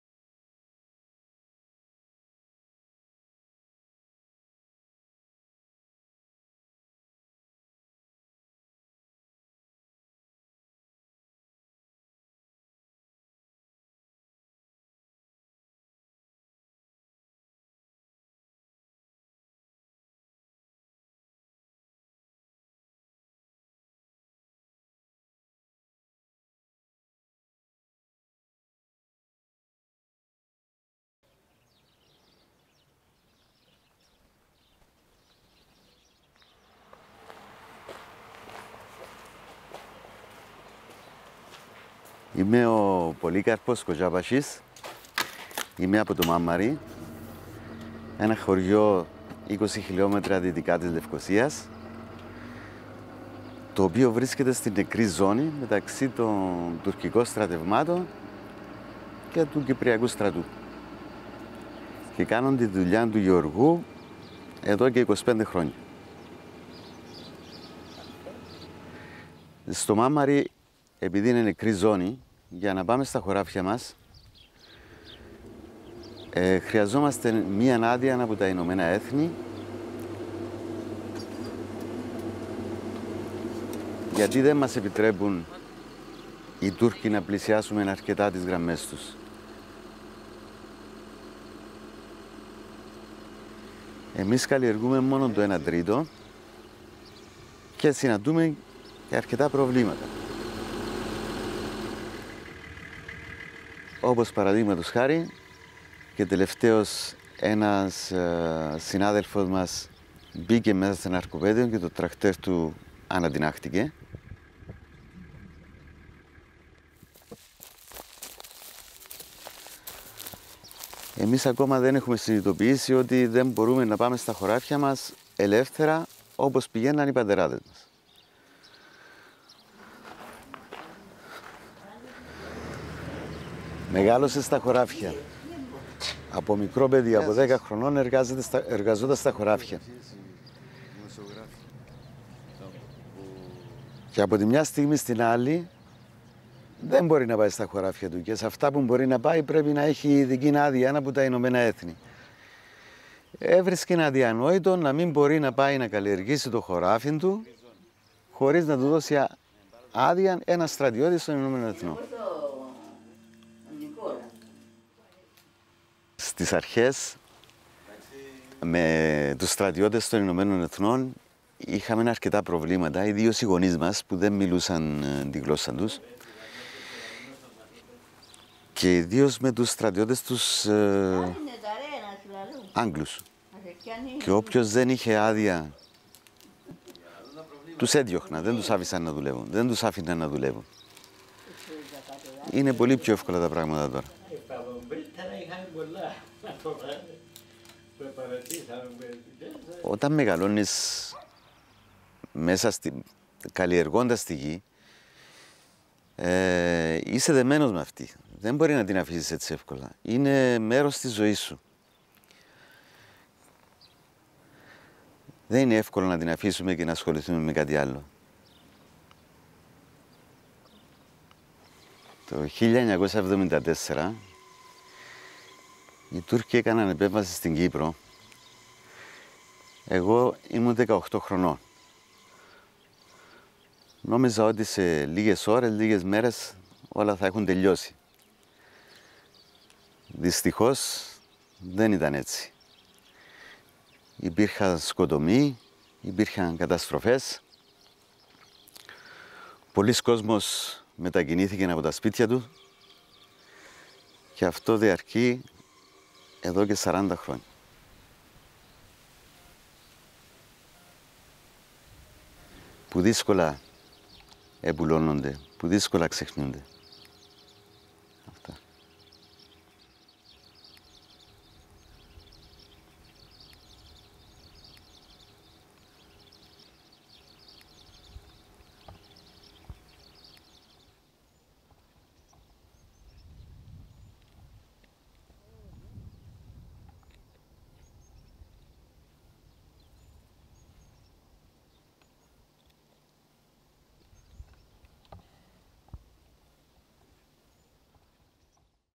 Minefields - Living In The Buffer Zone
Farmland is still cultivated inside the Buffer Zone. It cannot be done without danger however. Hear one farmers story.

Post Production: Elliot Graves